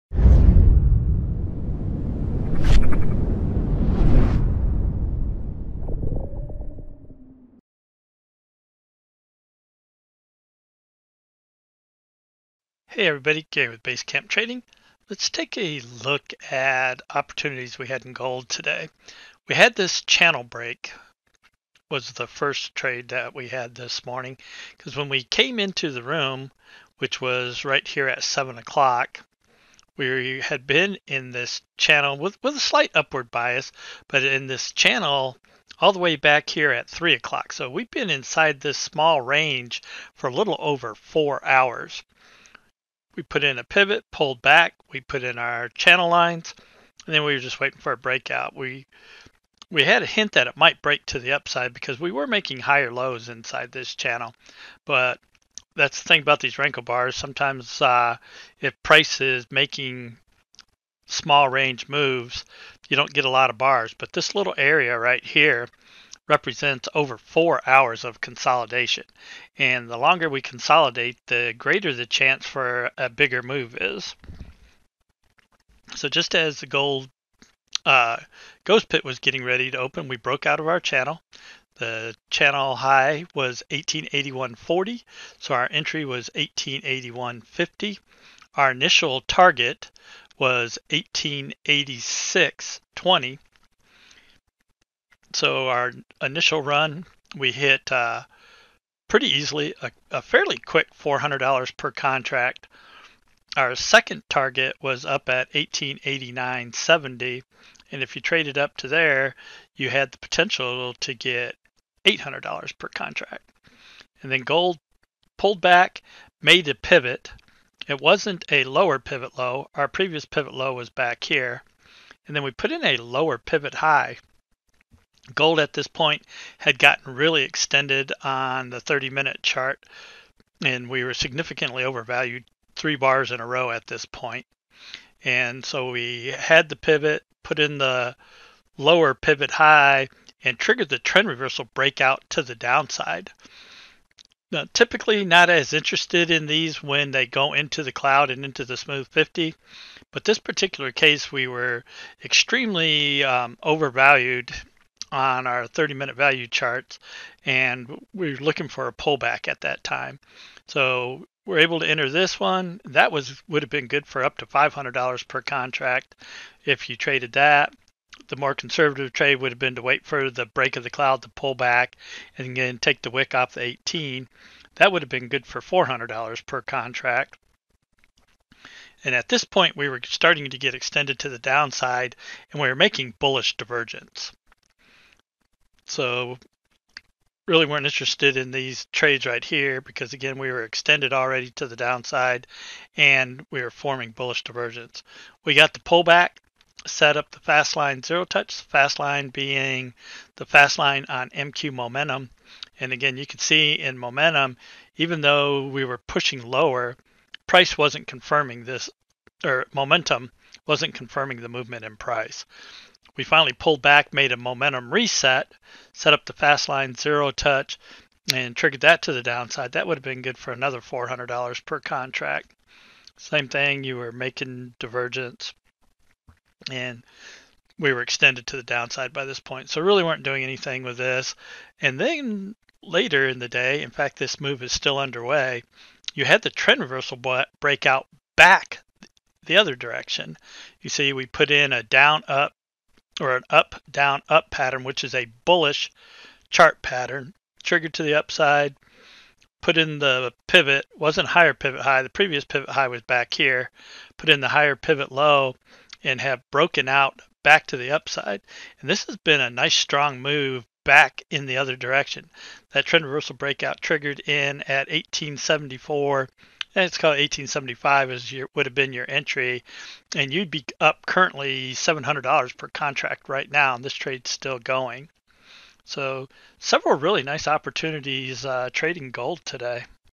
Channel Break In Gold
Channel Break In Gold. There several nice opportunities trading Gold Futures Starting with a compression channel break out.

Start your 30 Day Trial with BCT and Take Your Trading To The Next Level!